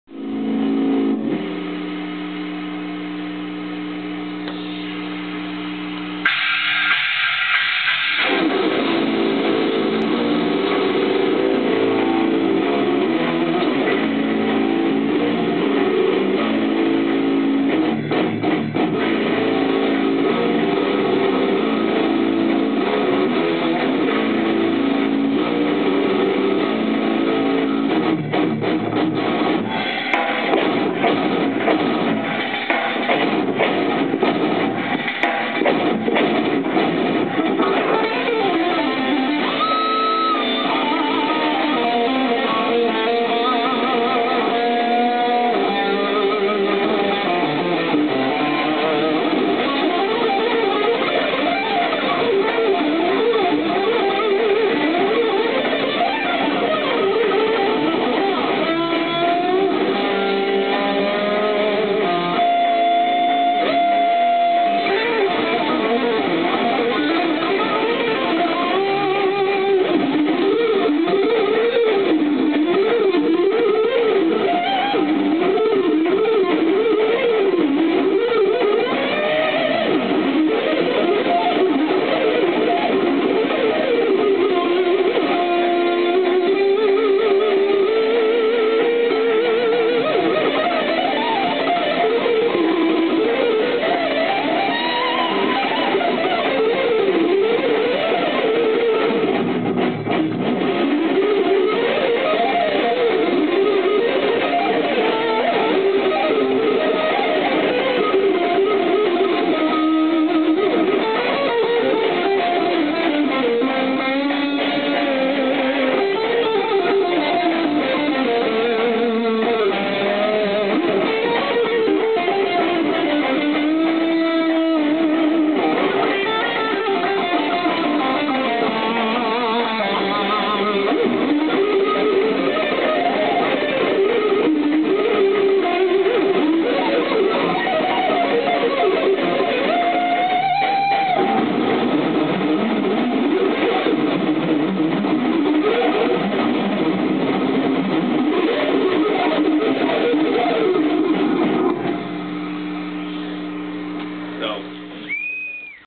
Chris Cannella (Jackson Guitars)
Chris Cannella (Austin, Texas Strait Music) Jackson Guitar Clinic.